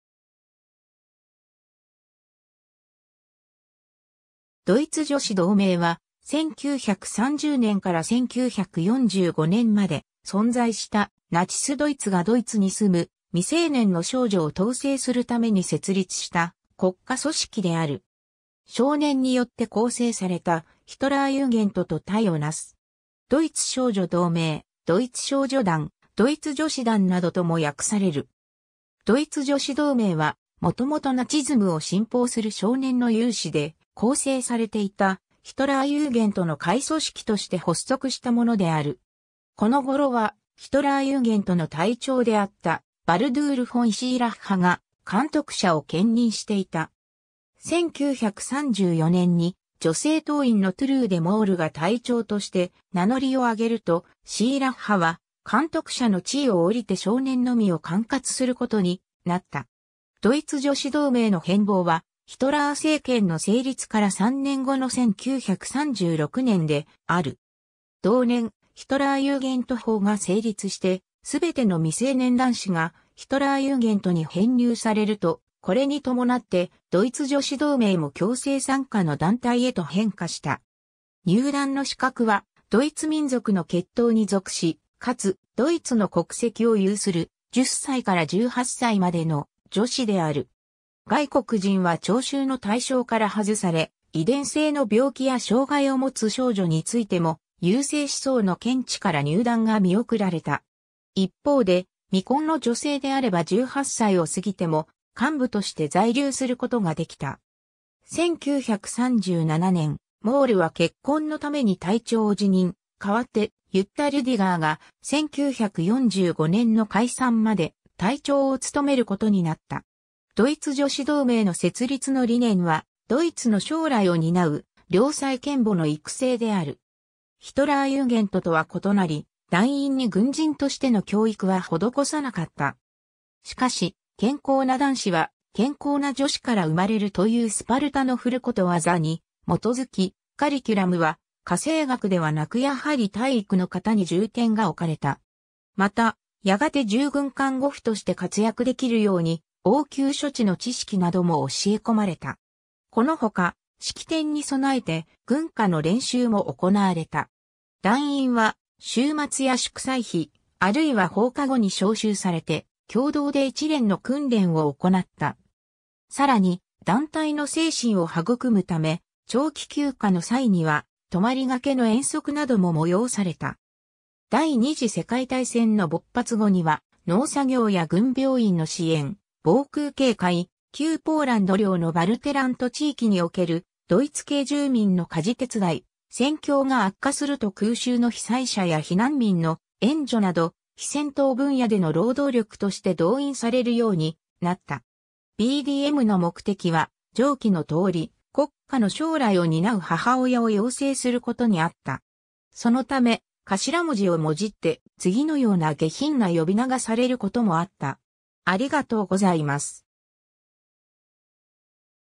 ドイツ女子同盟
ナチス・ドイツの社会
ドイツ女子同盟（ドイツじょしどうめい、独: Bund Deutscher Mädel、略称 BDM）は、1930年から1945年まで存在した、ナチス・ドイツがドイツに住む未成年の少女を統制するために設立した国家組織である。
少年によって構成されたヒトラーユーゲントと対を成す。
ドイツ少女同盟、ドイツ少女団、ドイツ女子団などとも訳される。
 ドイツ女子同盟は、もともとナチズムを信奉する少年の有志で構成されていたヒトラーユーゲントの下位組織として発足したものである。
この頃は、ヒトラーユーゲントの隊長であったバルドゥール・フォン・シーラッハが監督者を兼任していた。
1934年に女性党員のトゥルーデ・モールが隊長として名乗りを上げると、シーラッハは監督者の地位を降りて少年のみを管轄することになった。
 ドイツ女子同盟の変貌は、ヒトラー政権の成立から3年後の1936年である。
同年、ヒトラーユーゲント法が成立して全ての未成年男子がヒトラーユーゲントに編入されると、これに伴ってドイツ女子同盟も強制参加の団体へと変化した。
入団の資格は、ドイツ民族の血統に属し、かつドイツの国籍を有する、10歳から18歳までの女子である。
外国人は徴集の対象から外され、遺伝性の病気や障害を持つ少女についても、優生思想の見地から入団が見送られた。
一方で、未婚の女性であれば18歳を過ぎても幹部として在留することができた。
1937年、モールは結婚のために隊長を辞任、代わってユッタ・リュディガーが、1945年の解散まで隊長を務めることになった。
 ドイツ女子同盟の設立の理念は、ドイツの将来を担う良妻賢母の育成である。
ヒトラーユーゲントとは異なり、団員に軍人としての教育は施さなかった。
しかし、「健康な男子は健康な女子から生まれる」というスパルタの古諺に基づき、カリキュラムは家政学ではなくやはり体育の方に重点が置かれた。
また、やがて従軍看護婦として活躍できるように、応急処置の知識なども教え込まれた。
このほか、式典に備えて軍歌の練習も行われた。
団員は週末や祝祭日、あるいは放課後に招集されて、共同で一連の訓練を行った。
さらに、団体の精神を育むため、長期休暇の際には泊りがけの遠足なども催された。
第二次世界大戦の勃発後には、農作業や軍病院の支援、防空警戒、旧ポーランド領のヴァルテラント地域におけるドイツ系住民の家事手伝い（ポーランド系住民は追放されていた）、戦況が悪化すると空襲の被災者や避難民の援助など、非戦闘分野での労働力として動員されるようになった。
 BDMの目的は上記の通り、国家の将来を担う母親を養成することにあった。
そのため、頭文字をもじって次のような下品な呼び名がされることもあった。